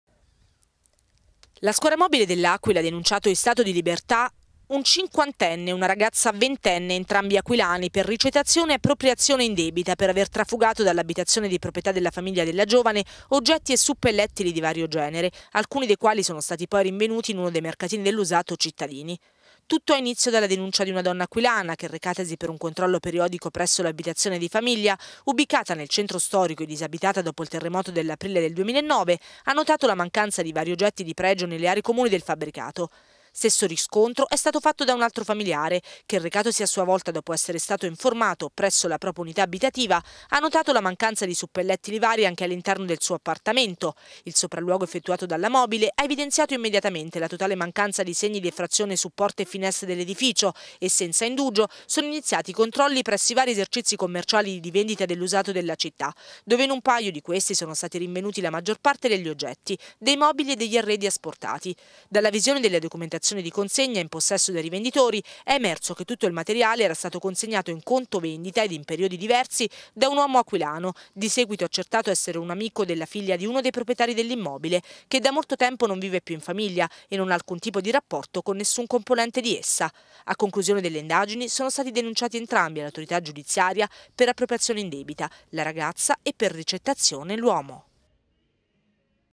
L'Aquila: svuotano casa disabitata, due denunce
Due aquilani sono stati denunciati dalla Squadra Mobile dell'Aquila per aver trafugato oggetti da una casa terremotata